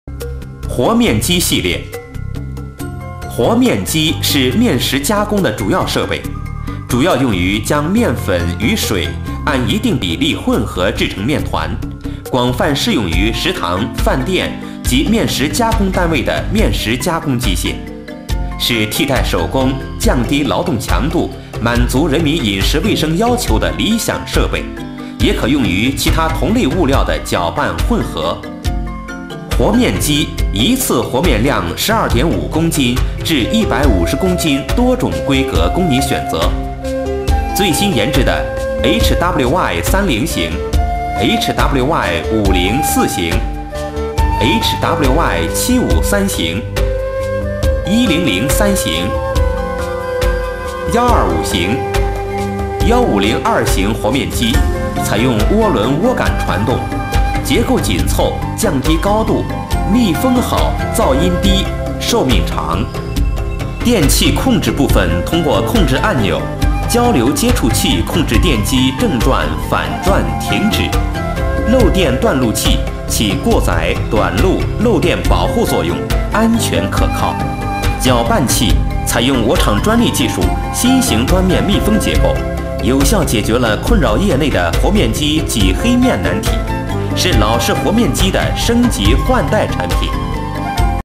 big capacity dough mixer machine 50kg 100kg 200kg
this heavy duty mixer is ideal for use in schools, bakeries, restaurants and canteens.
15kg, 20kg, 30kg, 50kg, 100kg and 200kg capatity for your choice.